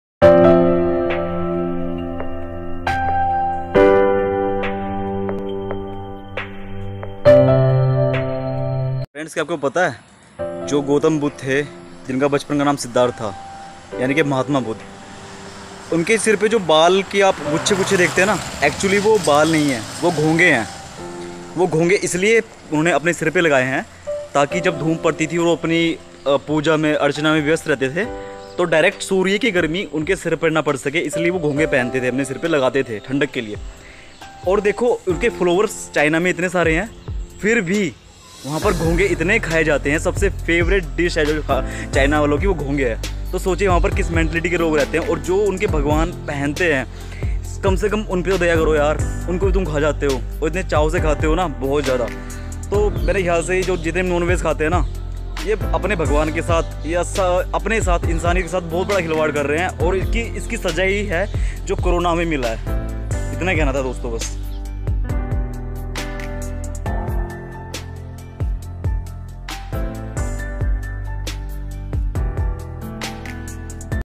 ये मेरे बाल नहीं है ये मेरे प्यारे 108 घोंघे हैं। मत खाओ इनको। lord Buddha hairs are snails
चाइना वाले सब कुछ खा जाते है
गौतम बुद्ध गर्मी ओर धूप से बचने के लिए घोंघे सनैल
बालो पर पहन लेते थे
चाइना वाले इस भी खा जाते है।
कुछ तो शरम करो।
चाइना मे ही सबसे ज्यादा बुद्धिष्ट लोग है जो
गौतम बुद्ध को फ़ॉलो करते है पूजा करते है उनकी

फॉलो my channel
Chota Doctor
like this vedio